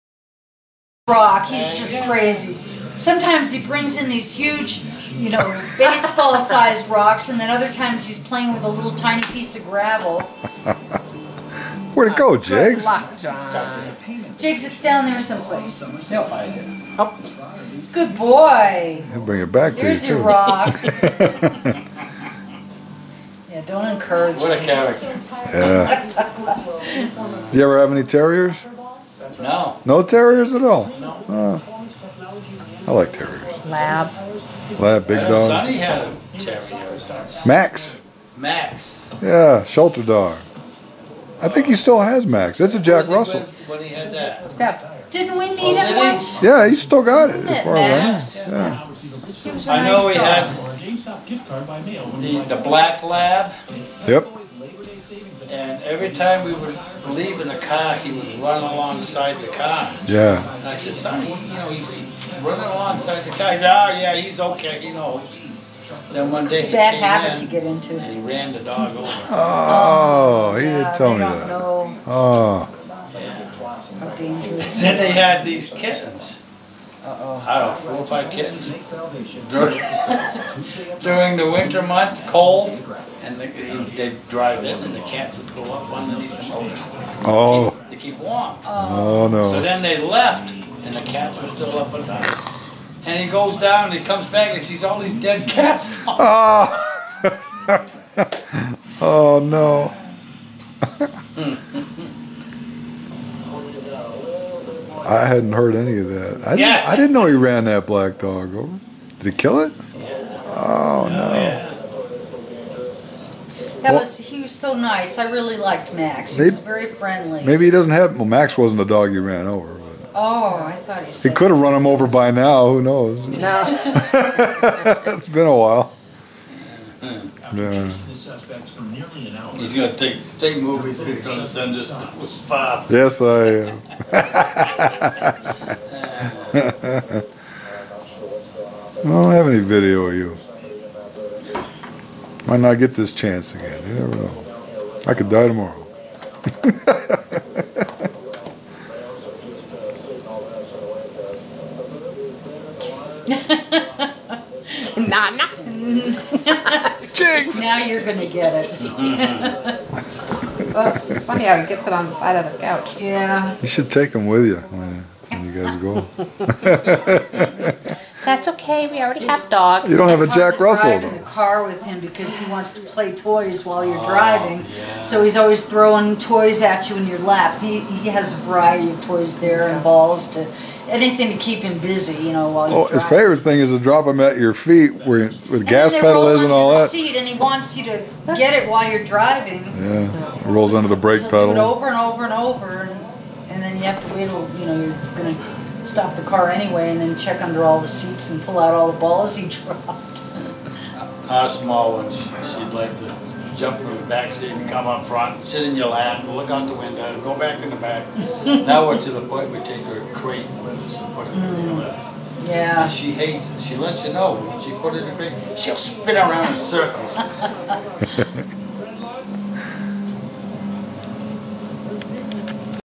2013 September 15th, Lou Ann and John Holt Visit Us In Their Camper For A Few Days Jiggs Is A Pest
My aunt and uncle had tried to drive down from Wyoming but a flood closed the highway and they had to turn back. They arrived the next day tired and frustrated, and then Jiggs wouldn't leave them alone. We spent the next few days showing them around Colorado.